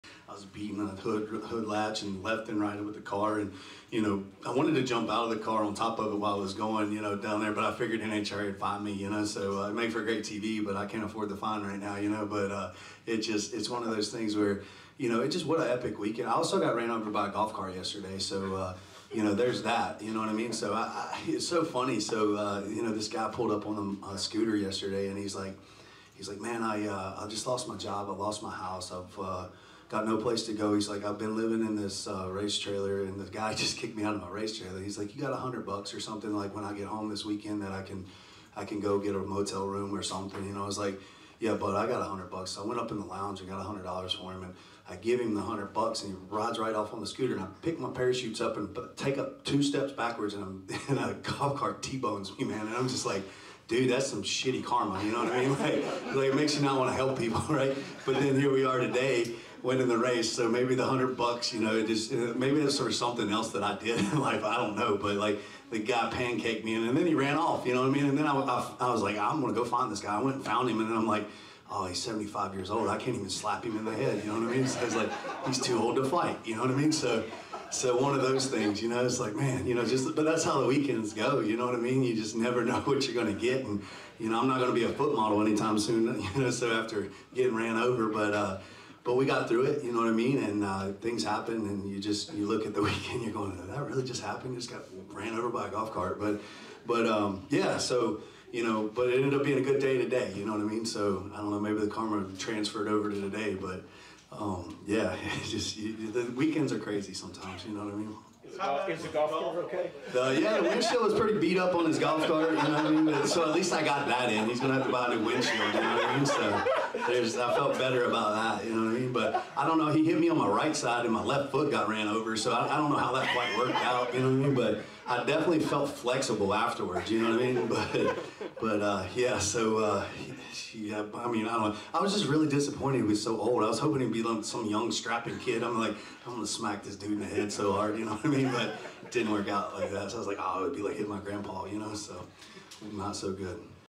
Matt Hagan Tells Story About Getting Hit By A Golf Cart During Dallas Weekend
Matt Hagan tells the story of how he got ran over by a golf court during the race weekend at the Fall Nationals.